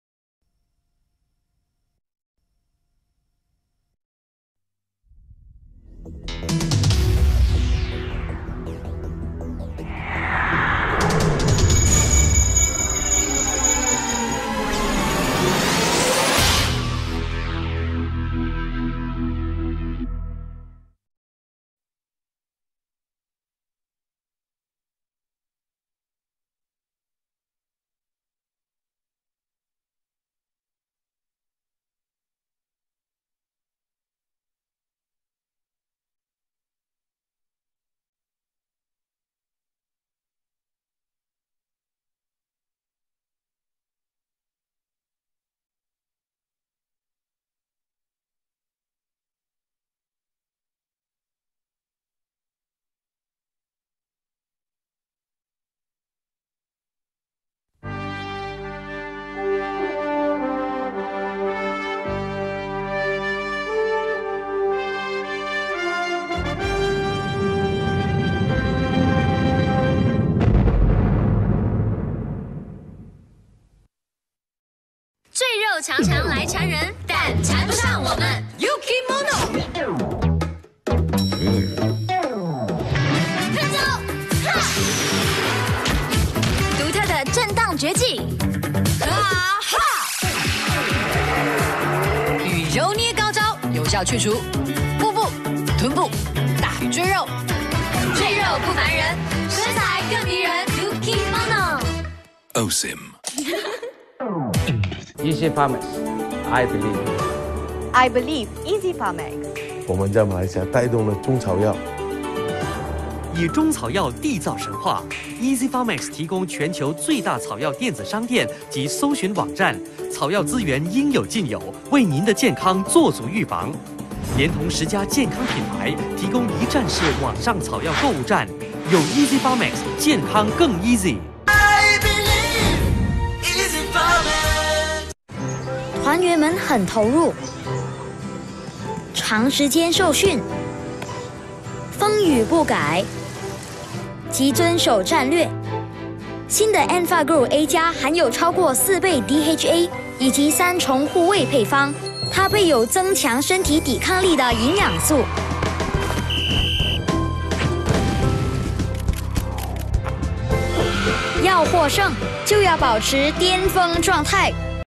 Multimedia, TVBI Logo, Warning, E-C, OSIM uKimono Commercial, Easy Pha-max Promo & Enfagrow A+ Promo
From "Born Rich" 2009 Malaysia DVD.